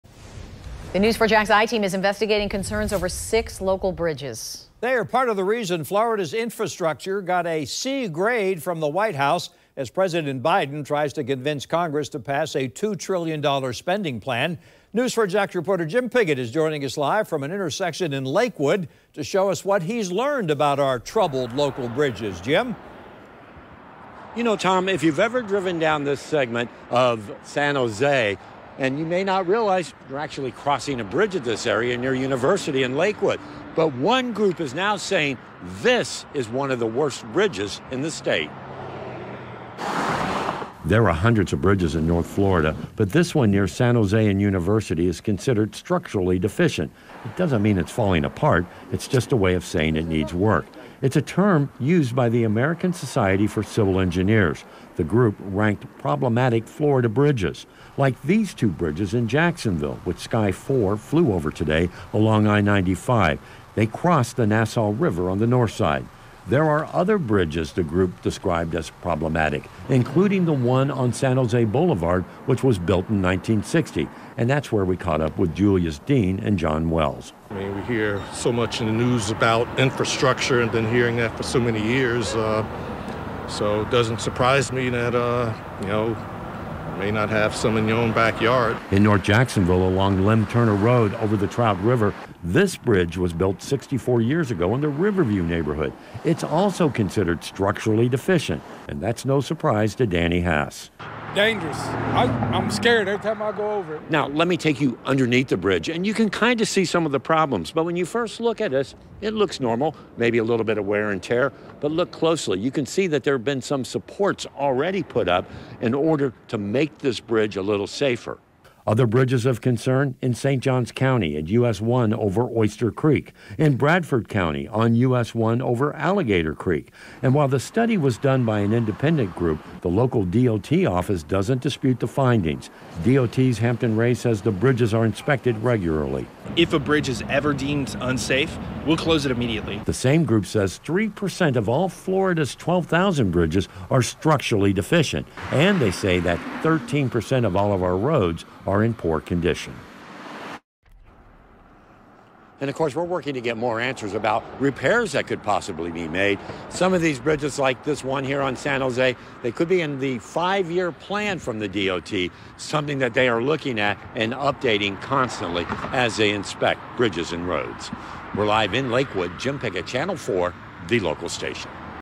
6 Jacksonville-area bridges ranked ‘structurally deficient’ by ASCE
A study by American Society for Civil Engineers, an independent contracting group, gives Florida’s infrastructure an overall grade of C- in its 2021 report card.